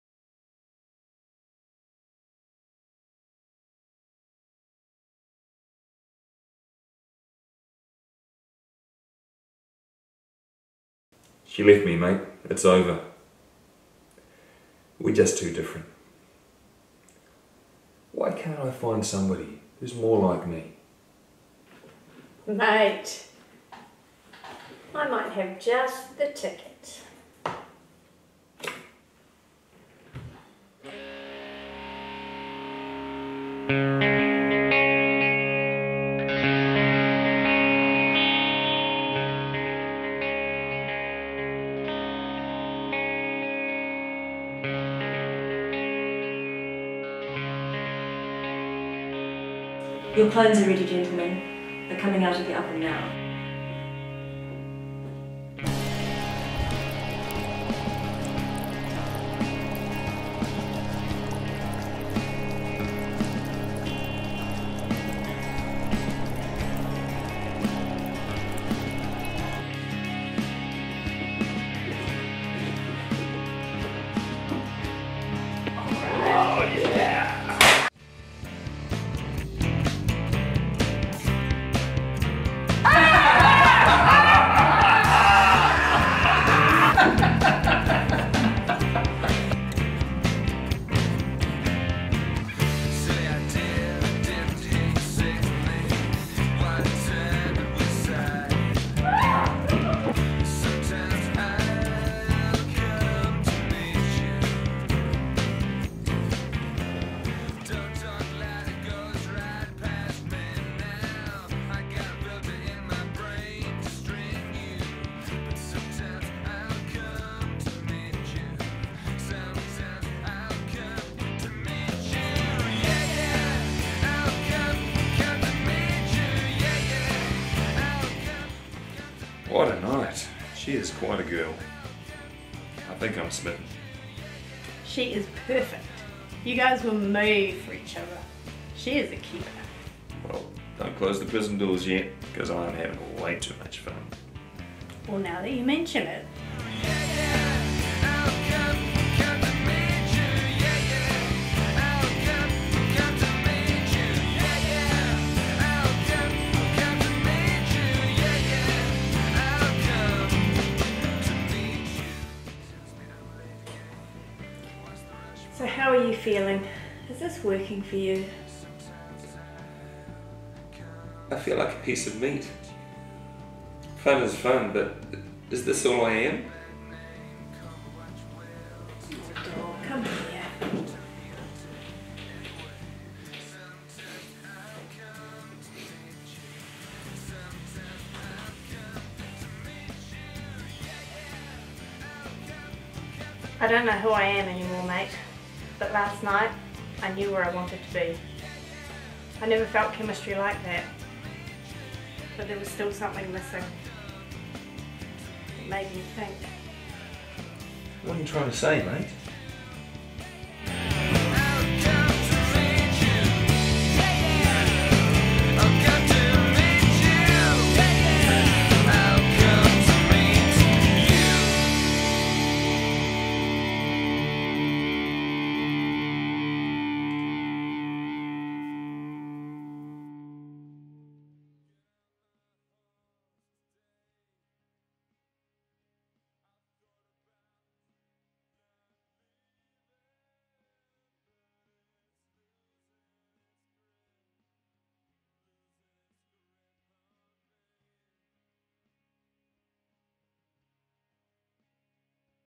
Clone Swappers
A short film made as part of 48 Hours 2019. Genre - Buddy movie. Elements - Wind, Double-take, Overhead Shot, Laughter